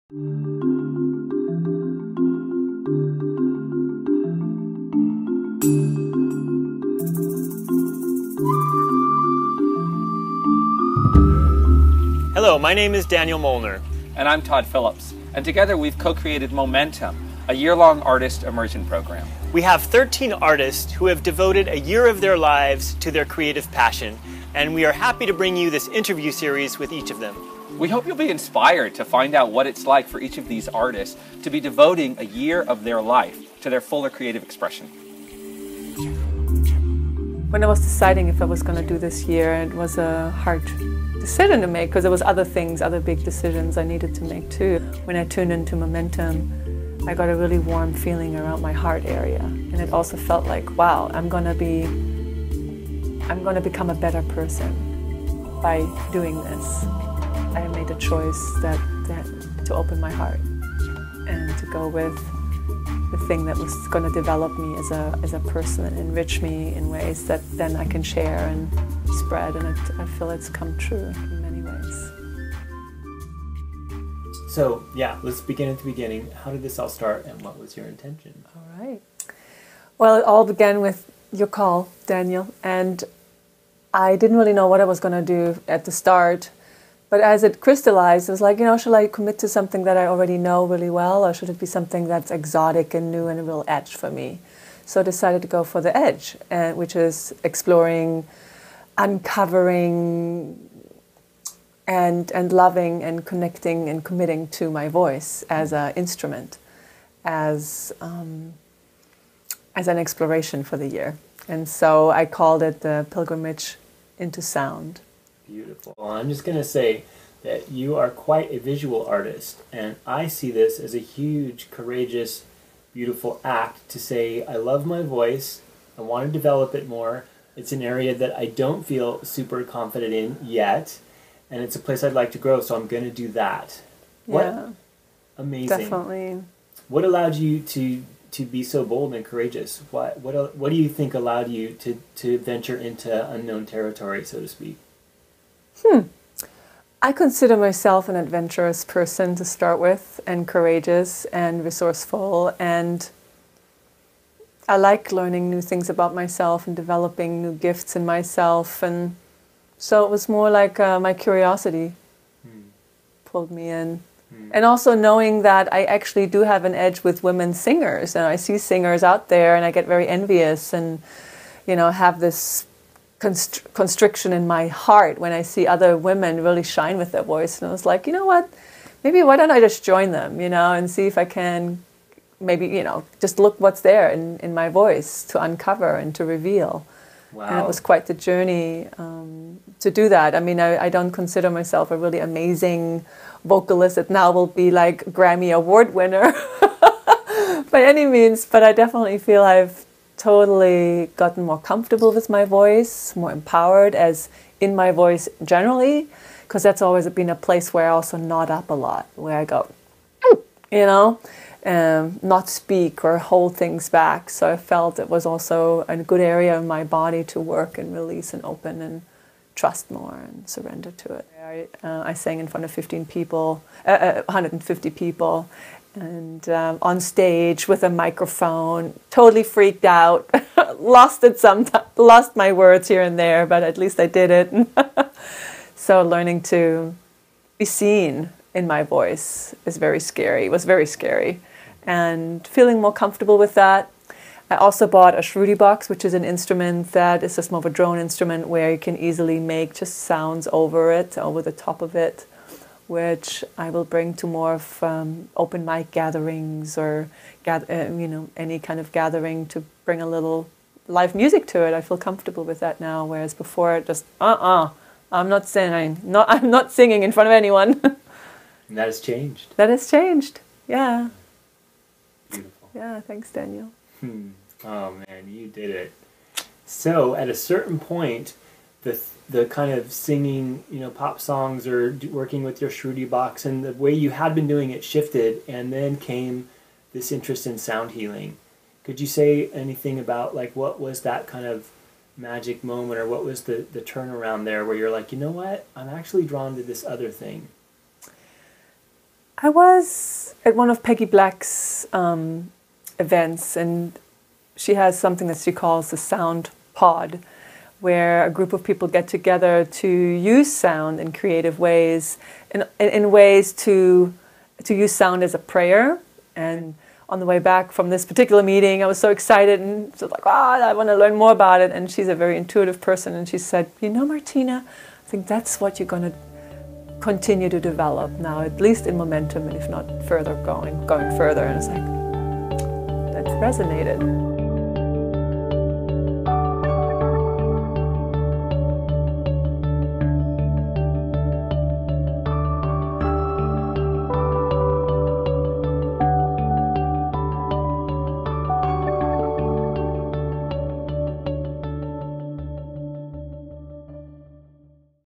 Martina Cornelius: Momentum 2014 Artist Interview Series
This video is about Martina Cornelius, Momentum 2014 artist. To see more of her work and for more information about the Momentum Artistis Immersion Program go